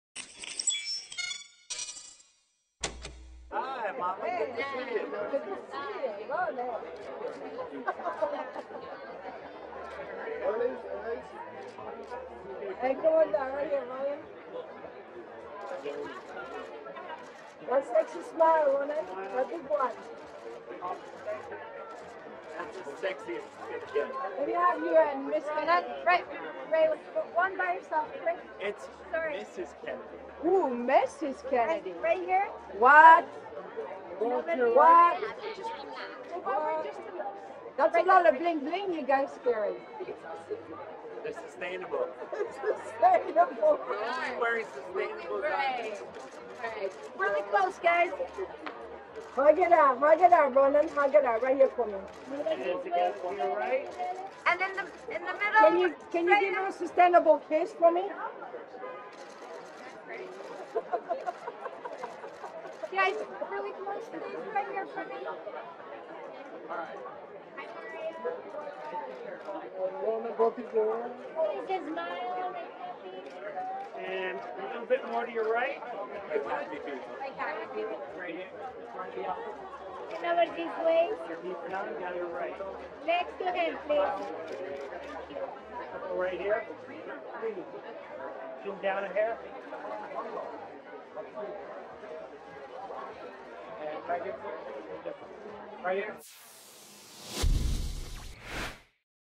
Ronen Rubinstein and Jessica Parker Kennedy 2022 EMA Awards Gala Green Carpet Arrivals
Broll footage: Ronen Rubinstein and Jessica Parker Kennedy on the green carpet at the Environmental Media Association (EMA) Awards Gala held at Sunset Las Palmas Studios in Los Angeles, California USA on Saturday October 8, 2022